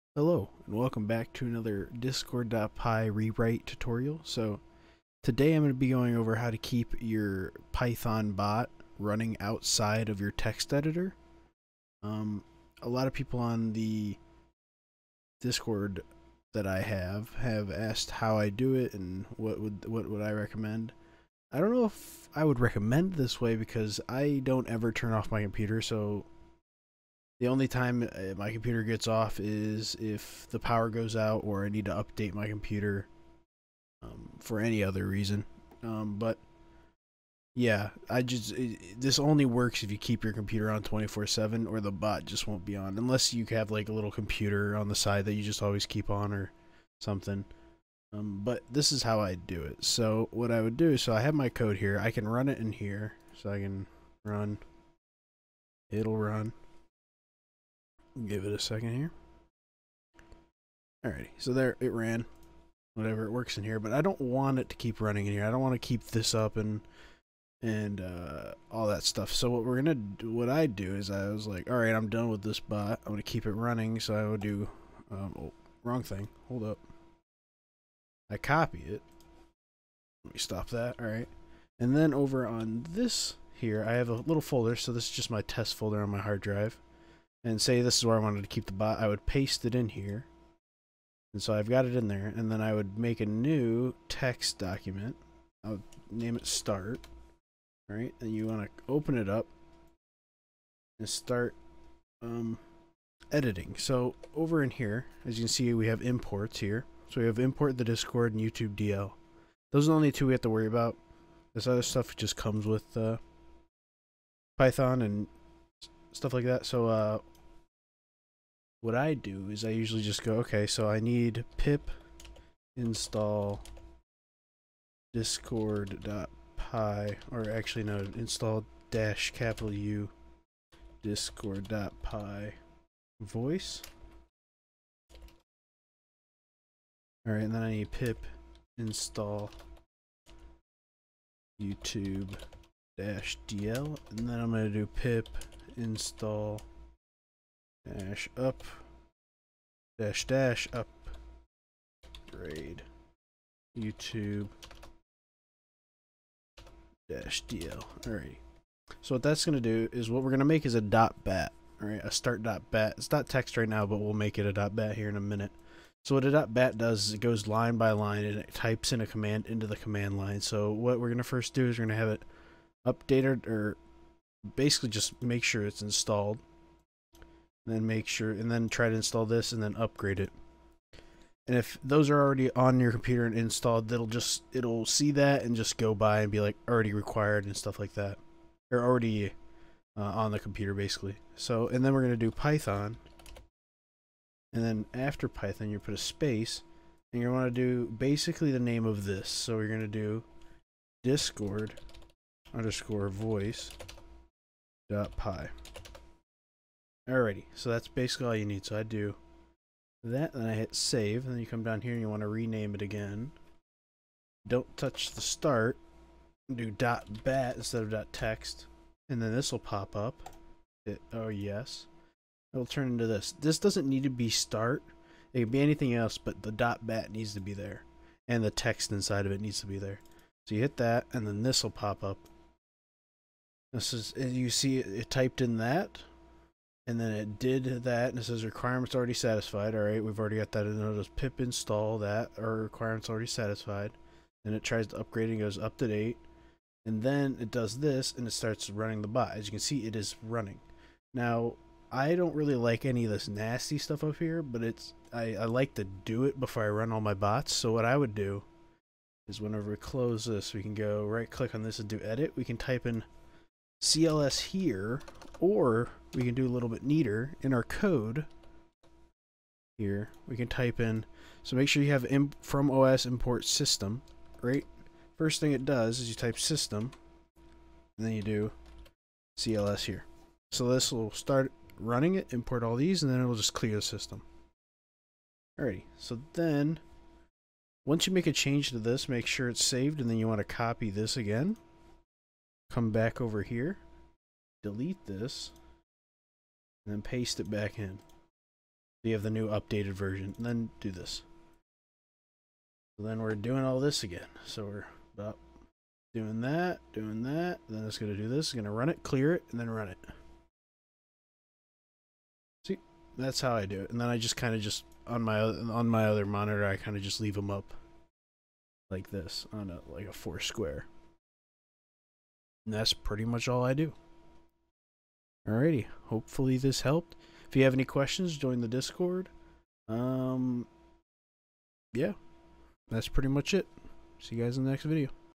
[Python]Discord.py Re-Write - Keeping the bot online outside of the IDE(Text-Editor)
This is a tutorial on how to keep the bot up and running outside of your IDE (Text-Editor) so you can do other things inside the text-editor without turning the bot off and on.

Warning: This only works when your computer is on, but once it is running it will stay running until the computer is turned off then have to be re-launched